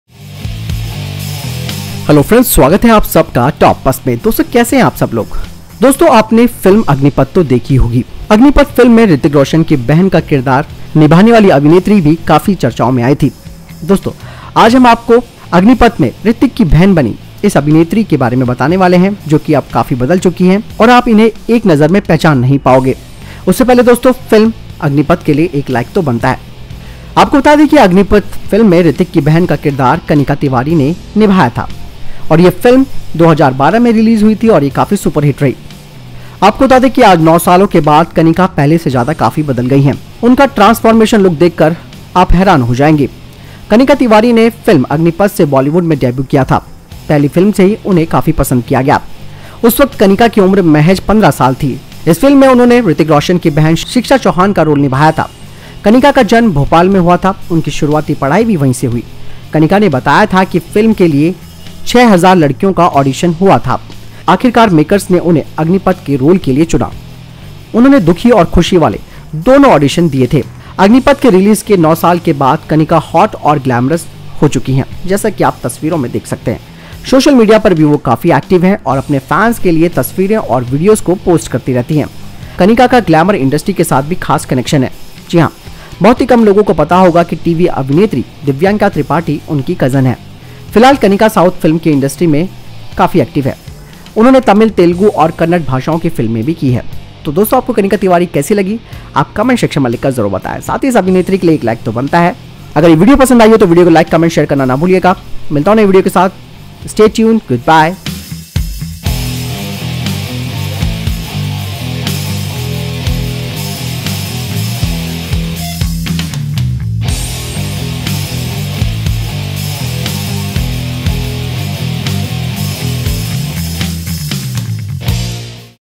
फिल्म अग्निपथ की ये बच्ची आज दिखती है गजब की। पहचान नहीं पाओगे। Actress Kanika Tiwari
Kanika Tiwari became Hrithik Roshan's sister in 'Agneepath', showed amazing transformation after years
The film 'Agneepath', made under the banner of Karan Johar's Dharma Productions, was a super hit at the box office. This film was released in 2012. It starred Hrithik Roshan, Priyanka Chopra, Rishi Kapoor and Sanjay Dutt in the lead roles. In the midst of all this, another character that caught everyone's attention was actress Kanika Tiwari, who became Hrithik's sister. After 9 years, Kanika has changed a lot already. His transformation look will surprise you.
Kanika Tiwari made her Bollywood debut with the film 'Agneepath'. He was well liked in the first film itself.
Kanika was 15 years old at that time. She played Hrithik Roshan's sister Shiksha Chauhan in the film.
Kanika was born in Bhopal. His early education also took place from there.
Kanika had told that 6 thousand girls were auditioned for the film.
Eventually the makers chose him for the role of 'Agneepath'. He gave both sad and happy auditions.
After 9 years of the release of 'Agneepath', now Kanika is seen in a hot and glamorous look.
She is very active on social media and keeps posting pictures and videos for the fans.
Kanika has a special connection with the glamor industry. Yes, very few people would know that TV actress Divyanka Tripathi is his cousin.
Currently, Kanika is very active in the South film industry. He has done films in Tamil, Telugu and Kannada languages.